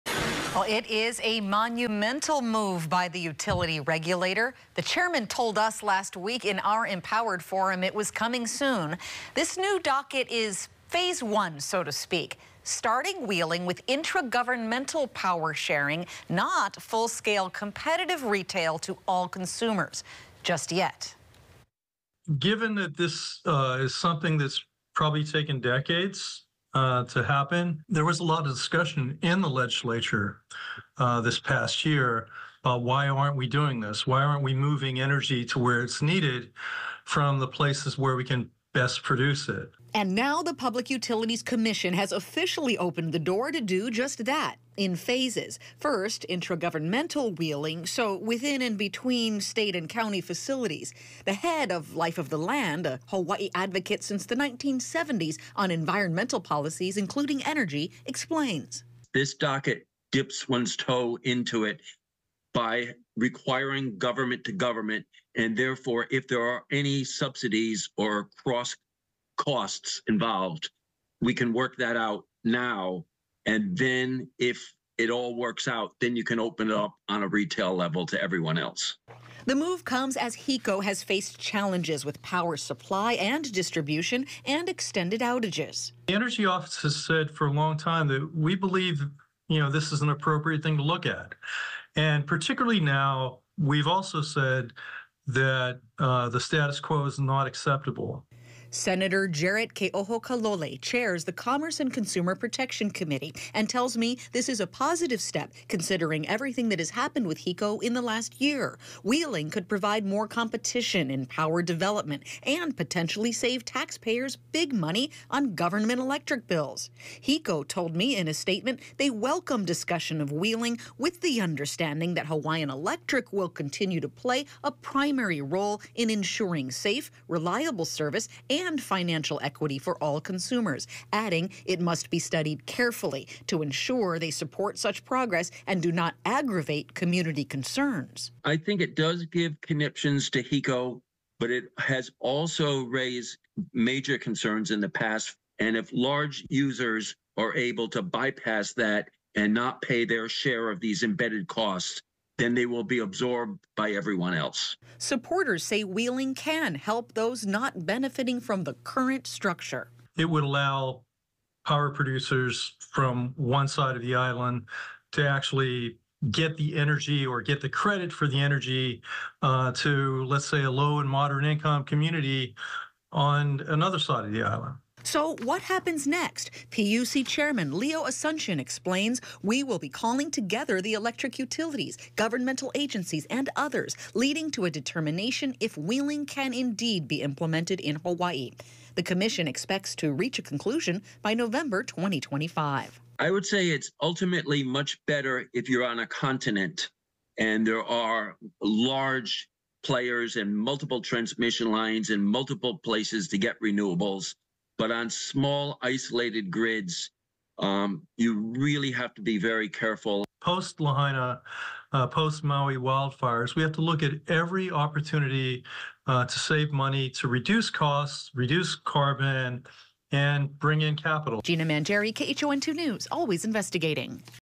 New wheeling docket could usher big energy market shift
The Public Utilities Commission has officially started looking into wheeling on Monday, which means new power producers could use existing utility lines to transmit electricity to other users.